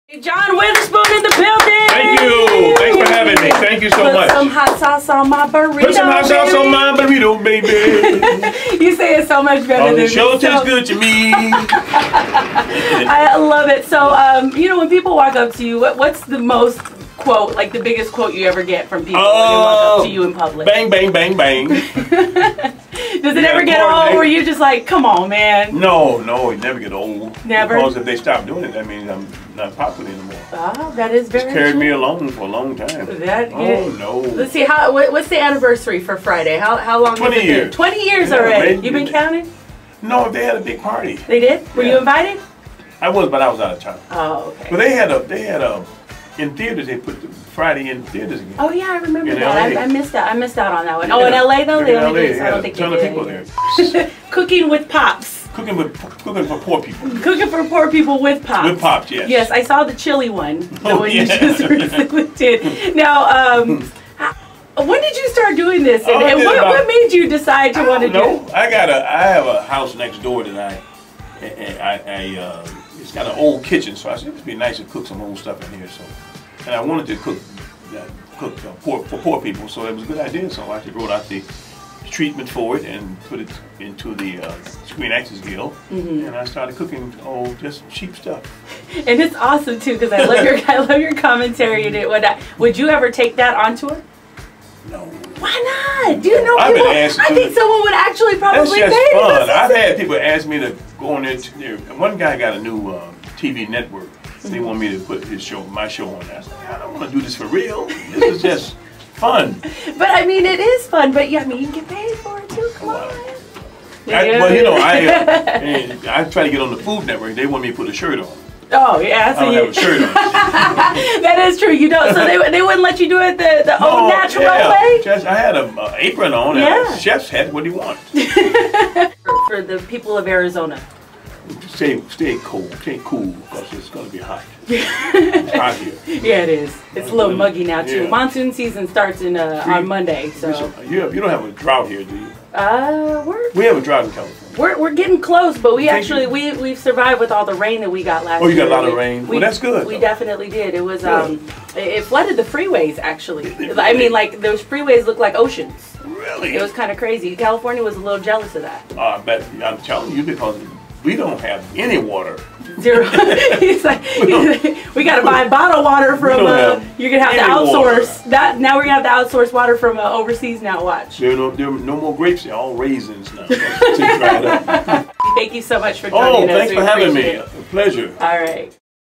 John Witherspoon interview w/ Nina Cruz Mega 104.3 JUNE 2015
John Witherspoon aka Pops stops by the station. Talks about how he loves his fans when they come up to him & use his quotes, still loves what he does. Also talks about his cooking show called Cooking for Poor People on his youtube page, Pops is a outstanding comedian & a great person.

Video By: Hector Arellano